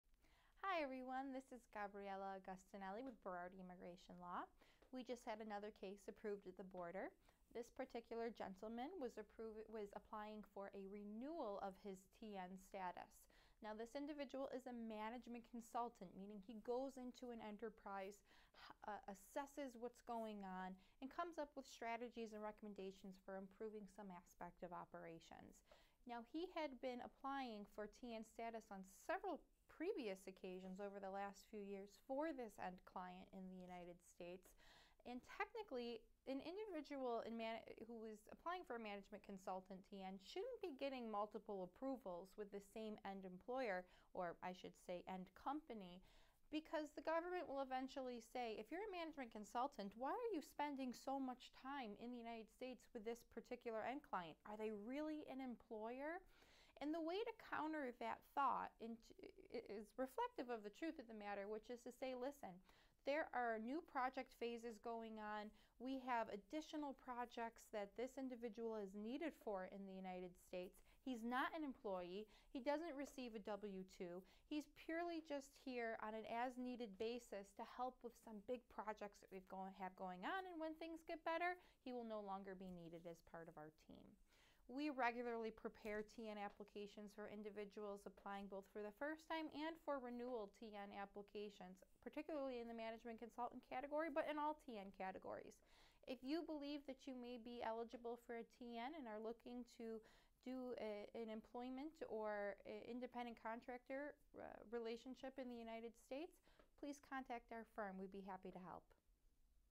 TN Renewal 01.02.2020
Our office regularly handles TN applications, both initial and renewal applications.  Attorney Gabriella Agostinelli discusses a recent success at the border for a TN Management Consultant.  The difficulty in the case was that the applicant held several previous TN visas, but Gabriella was able to show that he was not an employee and truly fulfilled the role of management consultant.